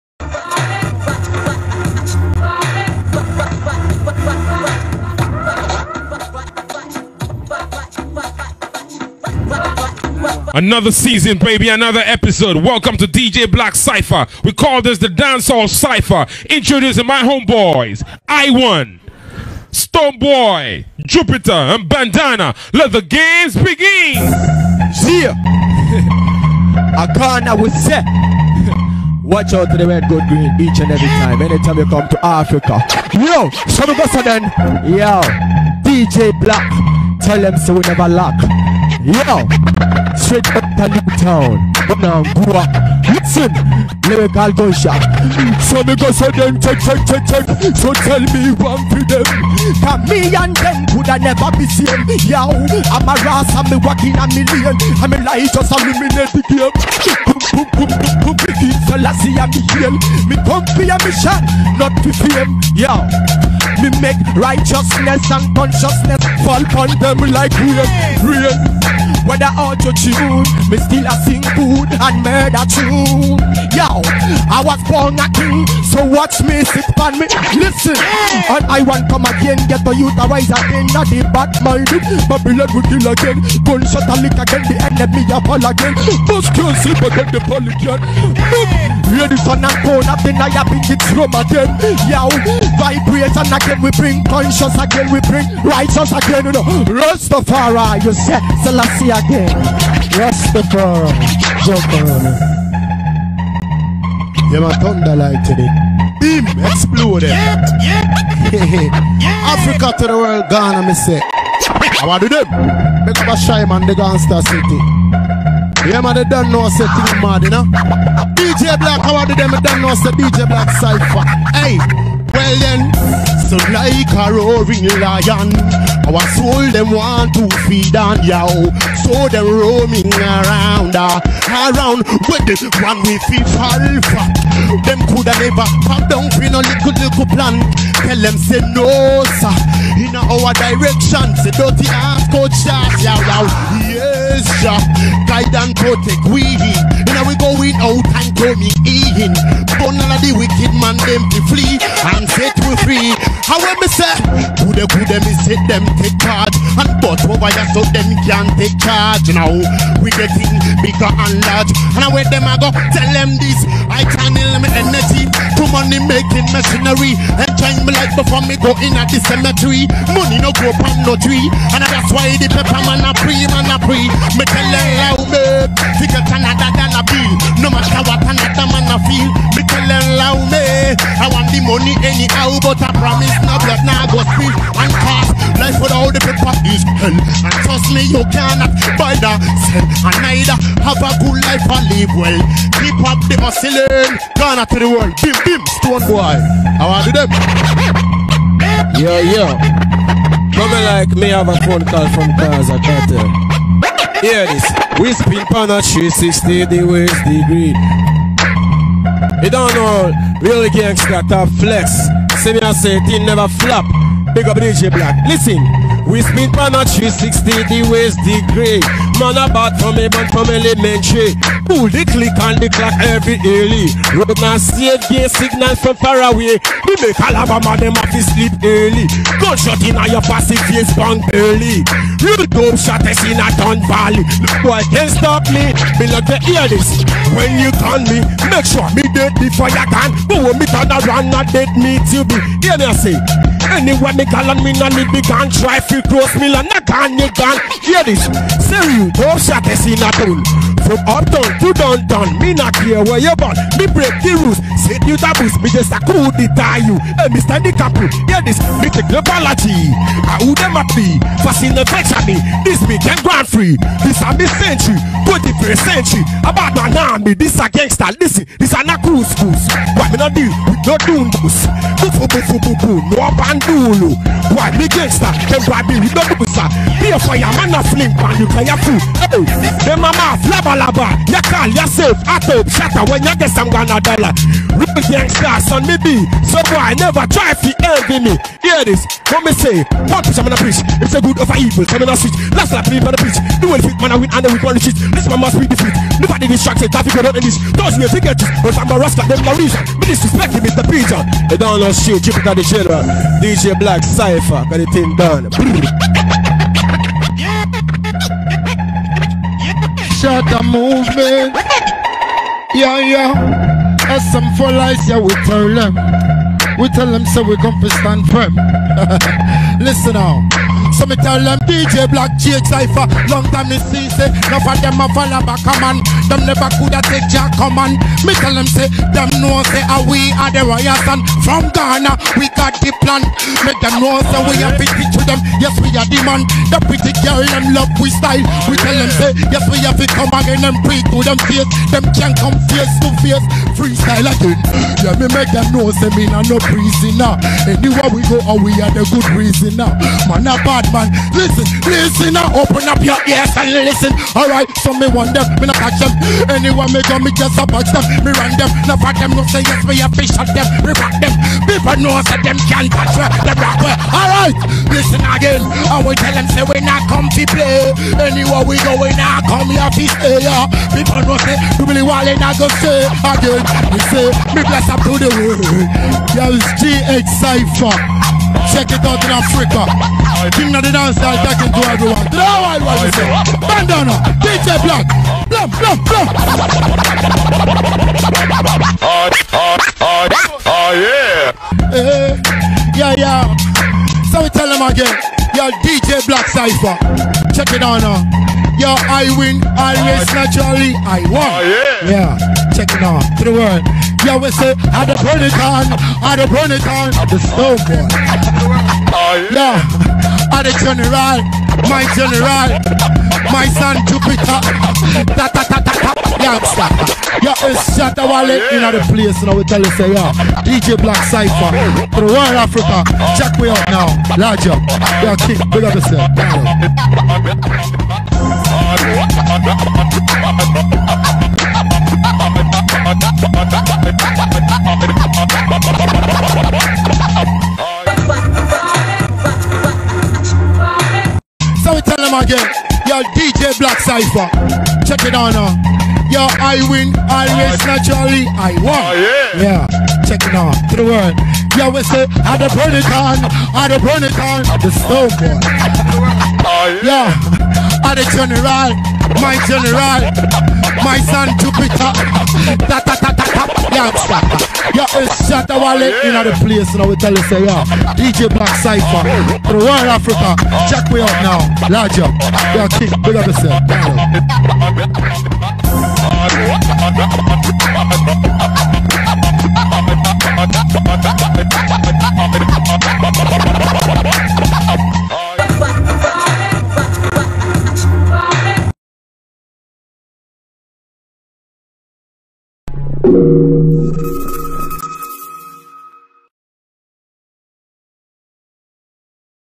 Freestyle Battle: Shatta Wale clash with Stonebwoy, Iwan, and Jupitar
Djblack's dancehall  Cypher feat. Iwan,Stonebwoy,Jupiter & ShattaWale.